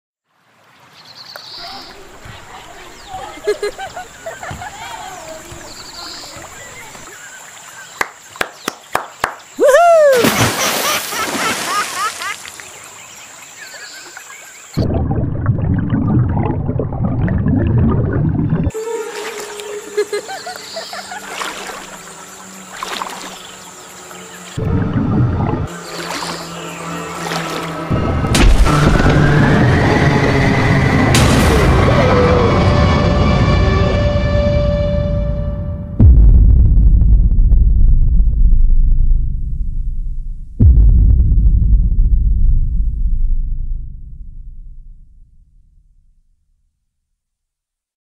BELOW.
A Short Animated Film I made as part of Project EVINC organized by the Leo Club of University of Moratuwa, Sri Lanka 😁✨

The theme of Project EVINC was "Evitiamo incidenti" - "Let's Avoid Accidents" and about the importance of preventing fatal accidents. I chose to focus my animation on Drowning and Water-related accidents 😌

Fun fact, the hand that touches the back of the boy's leg is actually not real but rather symbolism! It represents our curiosity, the "call of the void"  that acts as the root cause of many accidents.

Story & Animation by @benjinaraa (me lol) 😁✨
Created using Krita 🔥